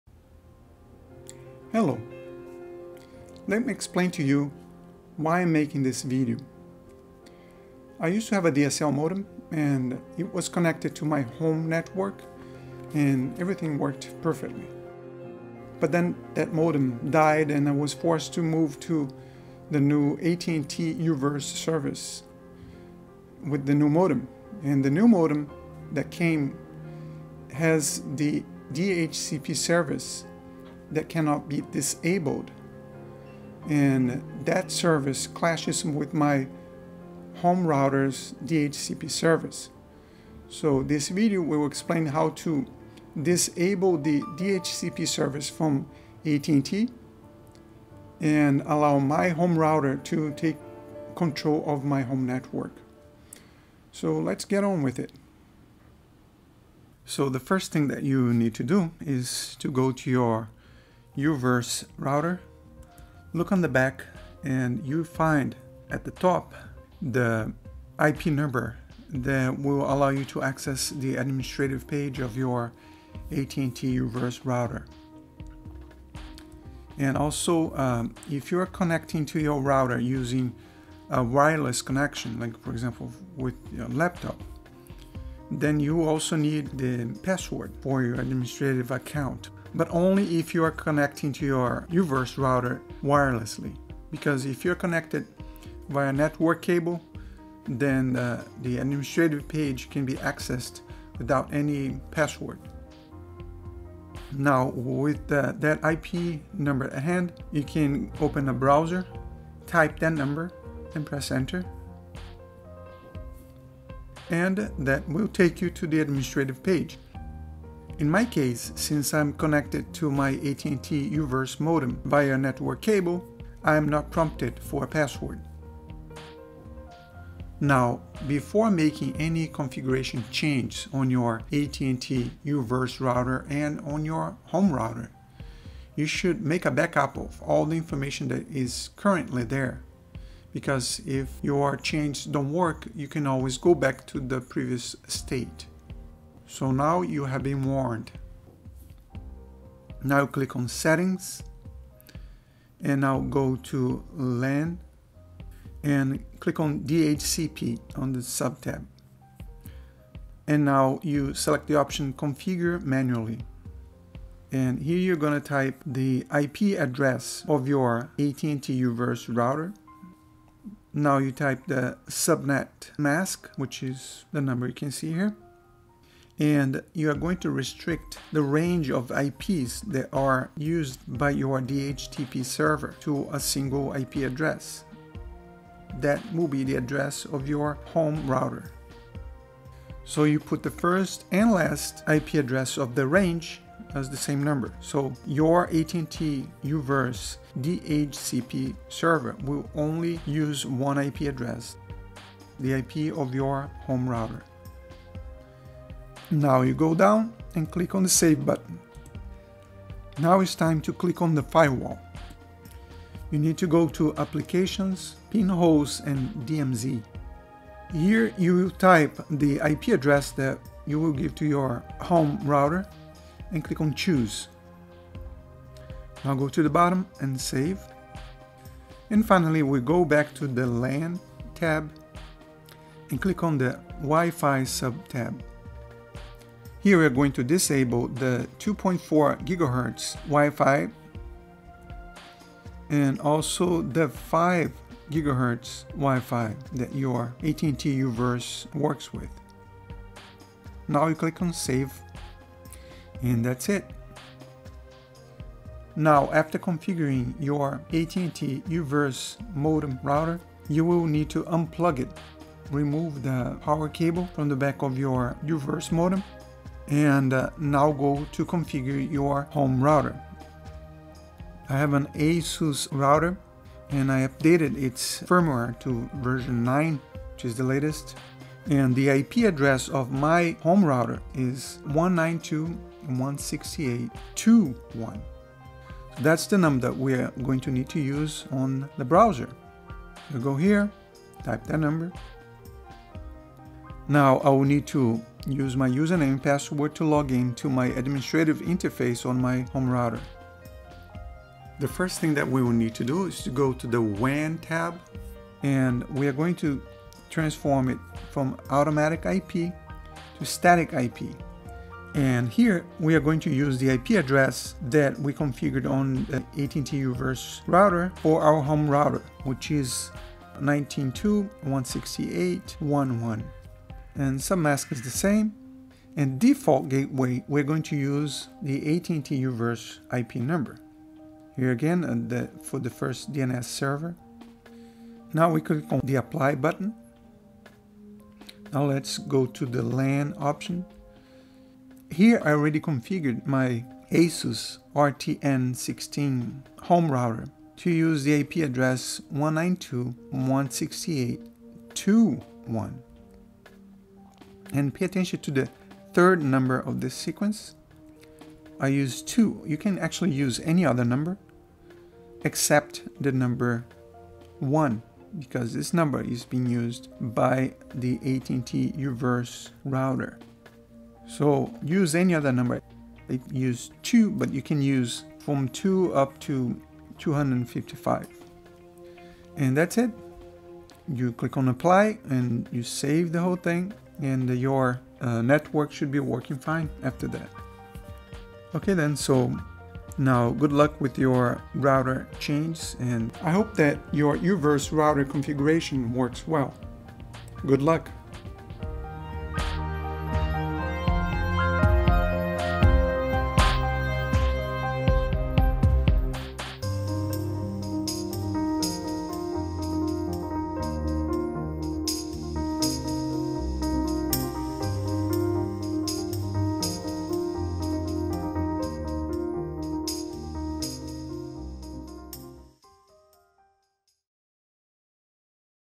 How to configure U-Verse to work with a home router
How to configure the AT&T U-verse modem/router to work with a home router.

The U-Verse router comes with the DHCP service enabled by default. That will create a conflict with your home router's own DHCP service. What makes things worse is that the U-Verse router's administrative page does not allow you to disable the DHCP service.

This tutorial will show you how to configure your U-Verse router to work in concert with your home router.

0:00 - Intro
0:56 - How to login to the U-Verse modem Admin page
2:07 - Backup the U-Verse modem settings
2:29 - Disable DHCP on U-Verse router
4:32 - Configure ASUS RT-N16 to connect to single IP
6:32 - Final considerations